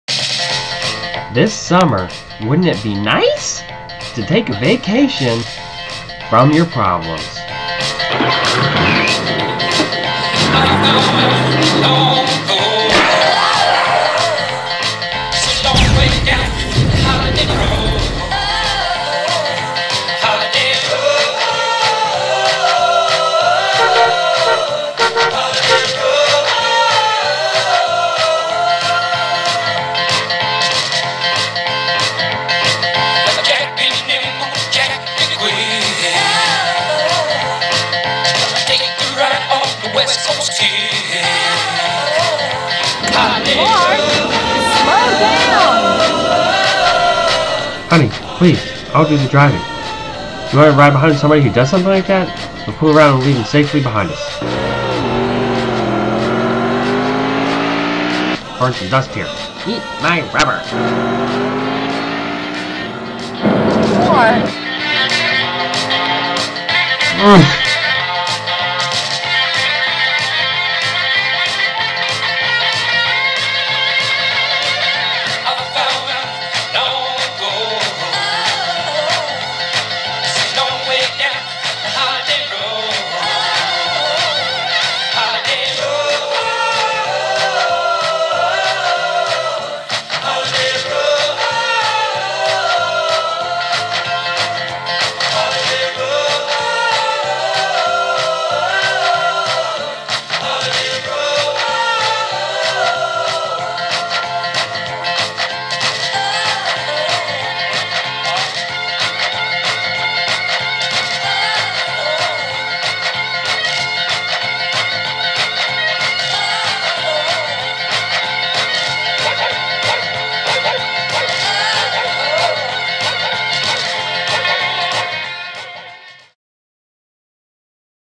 Camping "Vacation" spoof
This is a little video of my family gone camping.. "Vacation" style!  This video wasn't really planned out to much and i know the acting isn't to great. but i just wanted to have fun with it! More serious videos are in the works...  Hope you enjoy it!